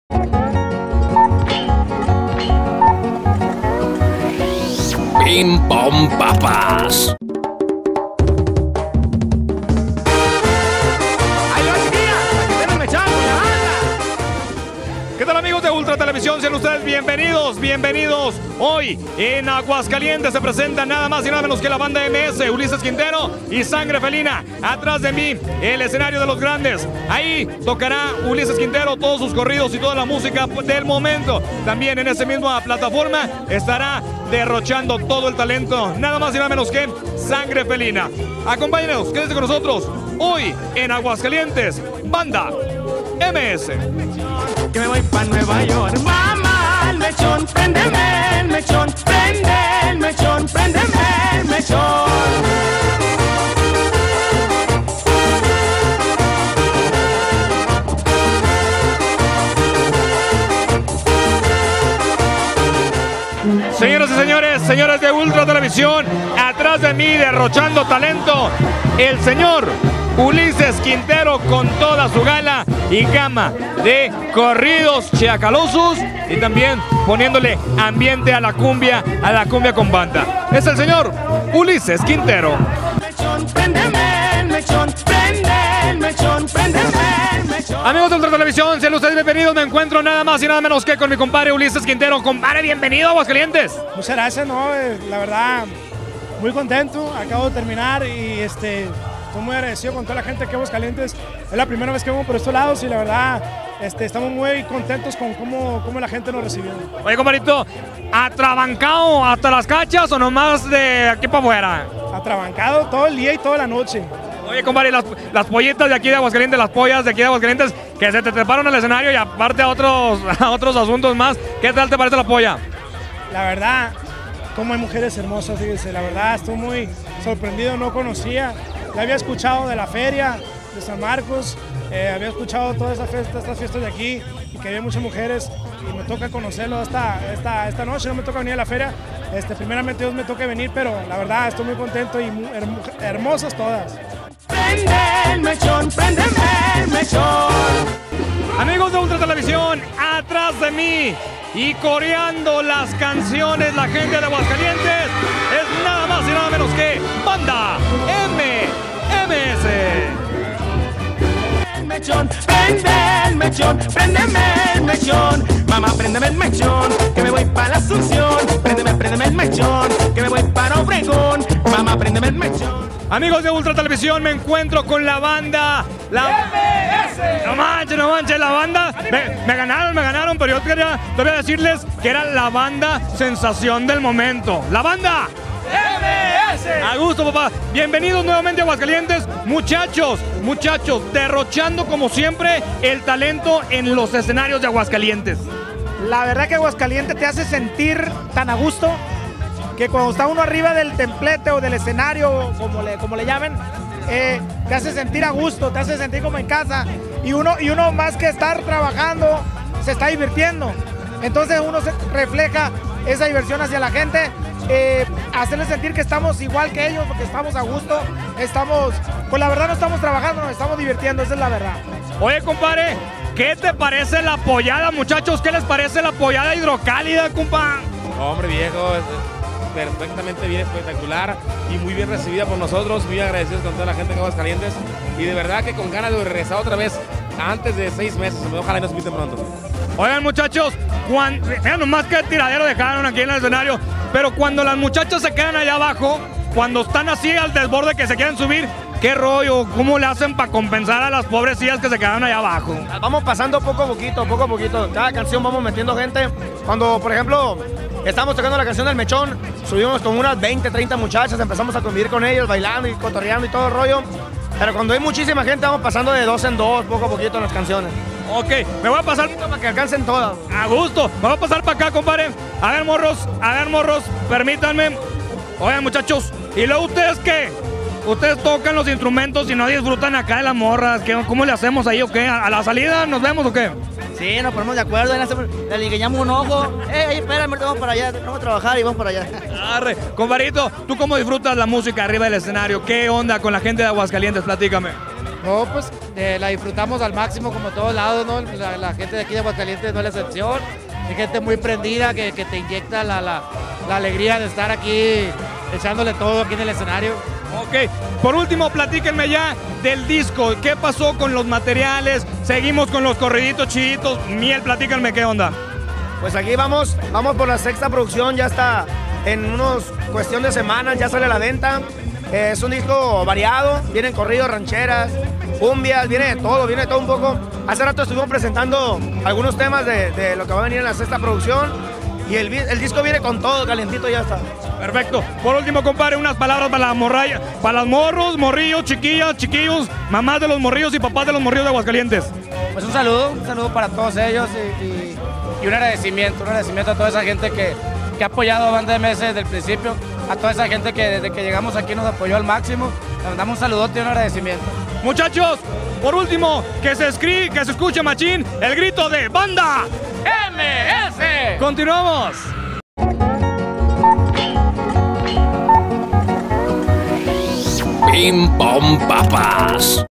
banda MS prendio el menchon y a toda la raza de aguascalientes
Ulises Quintero, Sangre Felina y Banda MS le pusieron todo el talento musical a la explanada de la villa charra, la cual se abarroto con mas de 8 mil personas